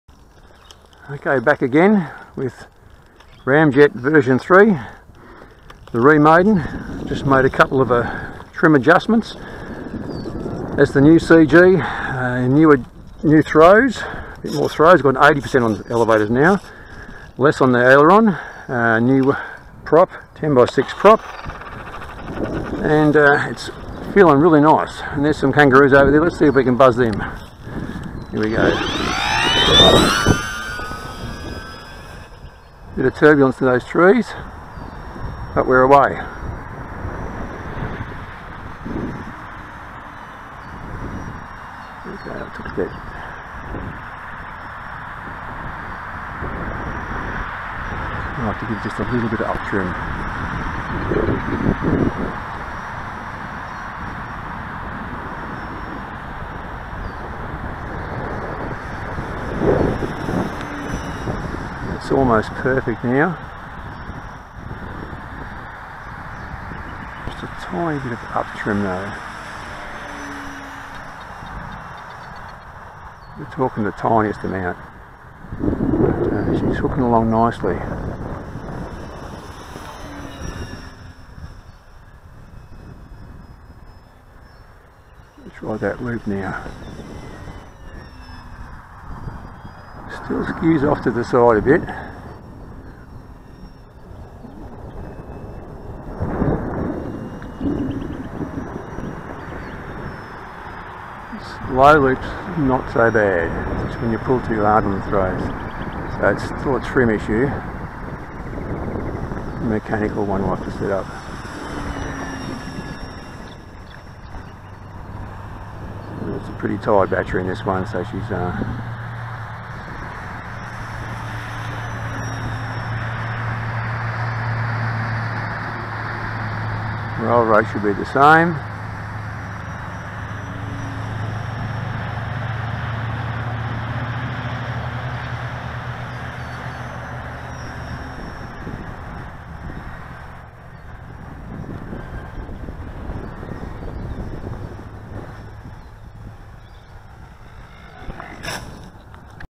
Ramjet-V3 Custom FPV Wing - Re maiden with Shorter Nose...
This is my custom build wing 'Ramjet-V3'.  This is the 3rd version & after today I feel like I have nailed the setup.  The original 'Ramjet' had a much longer nose, but it was affecting the flight performance, mostly when turning, straight line stability was excellent...  I first shortened the nose by 100mm, then re-tested...  it was getting better but not perfect, so I have shortened the nose again another 50mm for 'Ramjet-V3' & I think this one is by far the best...  Still a couple of small tweaks to make it better, but it's flying very good here today...  :-)  Now only 650 grams flying weight & super quiet !!